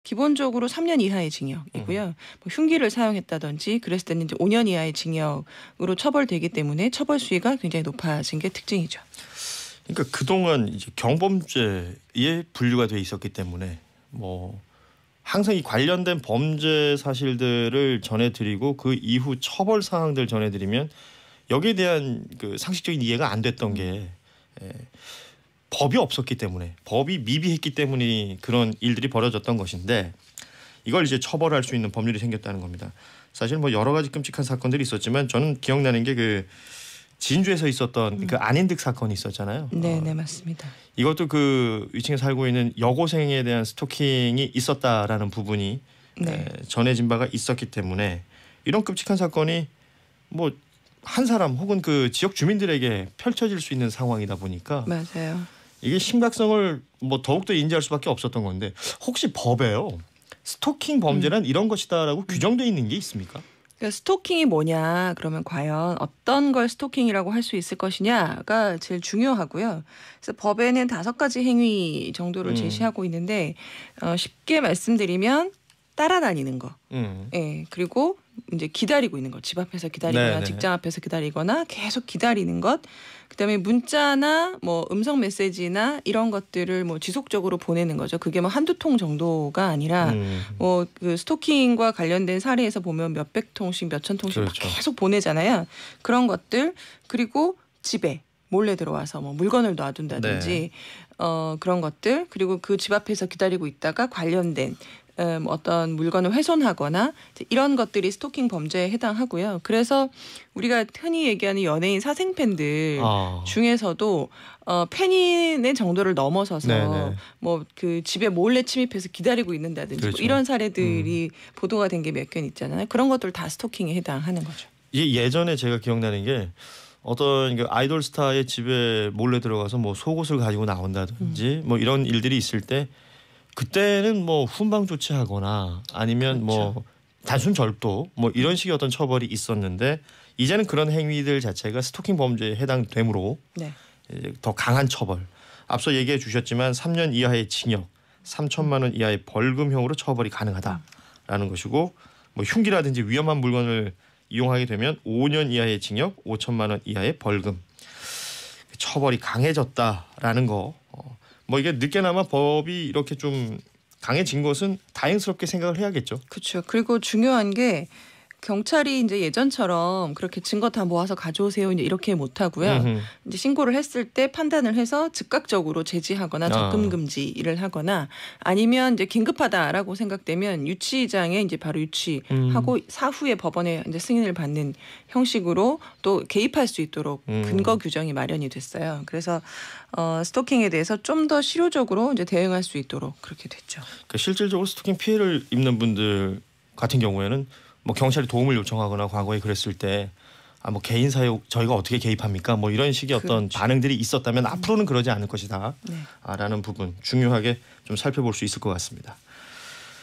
[고현준의 뉴스 브리핑] 22년 만에 '스토킹 범죄' 처벌 길 열렸다
*2021년 3월 30일* 고현준의 뉴스 브리핑